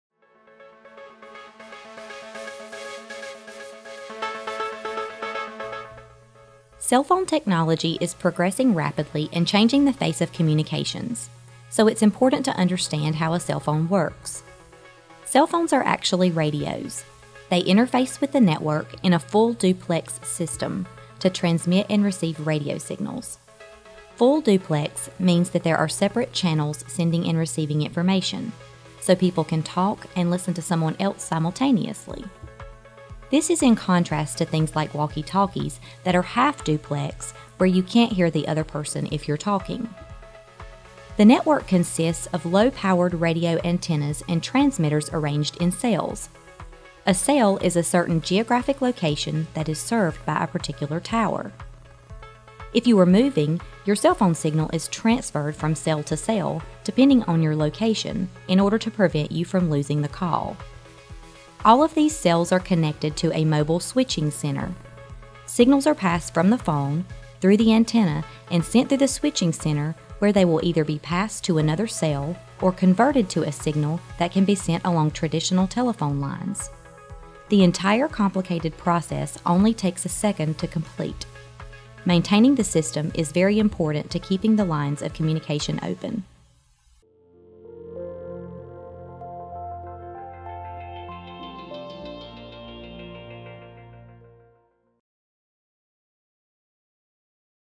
How do Cellphones Work?
Cellphone technology is progressing rapidly and changing the face of communications. So, it’s important to understand how a cellphone works. This video guides you through the basics of how mobile phones operate.